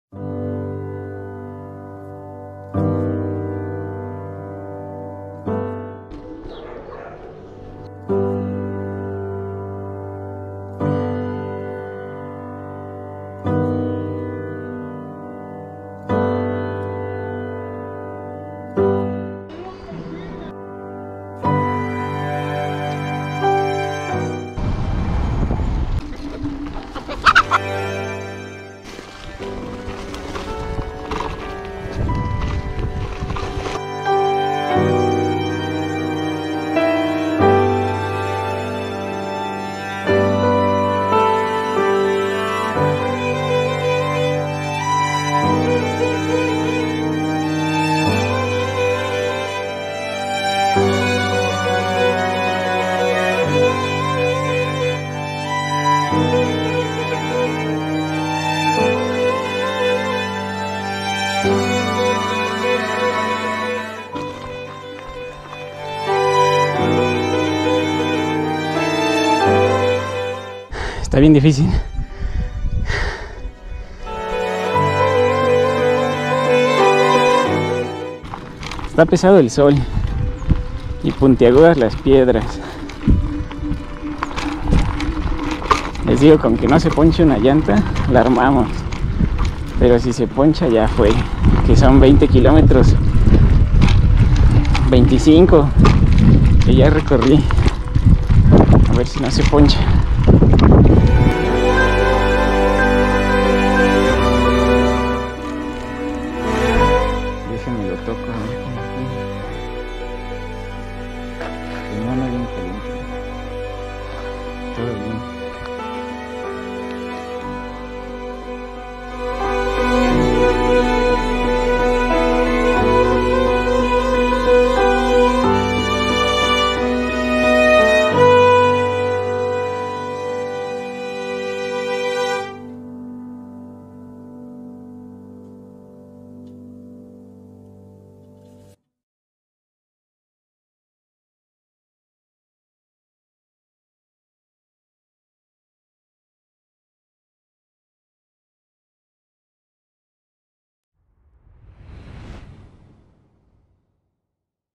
Subiendo una montaña en Scooter Eléctrico Patin Blade GT
Scooter: Blade GT 23AH 3000 watts